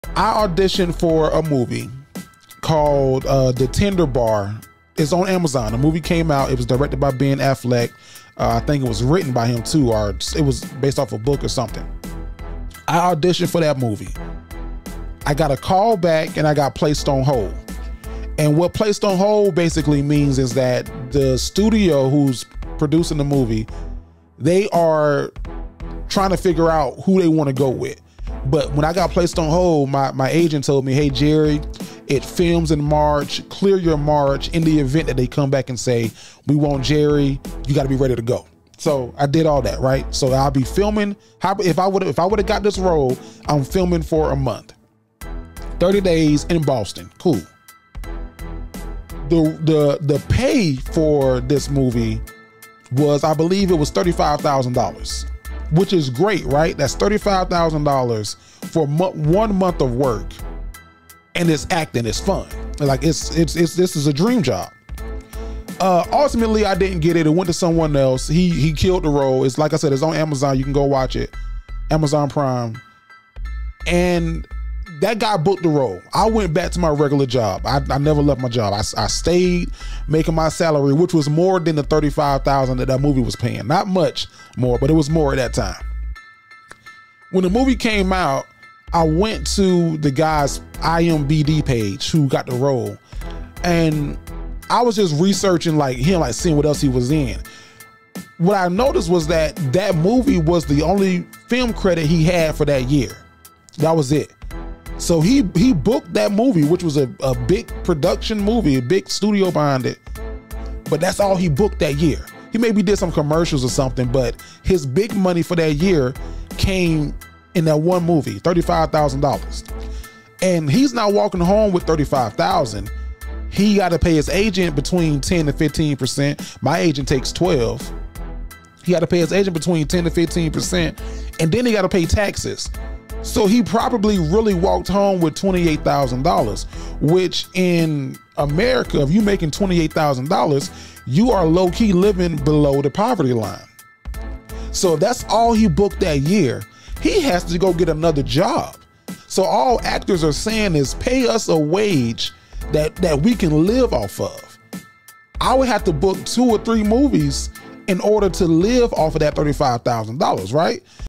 When they talk about paying actors, it's because of this!
When they talk about paying actors, it's really because of reasons like this!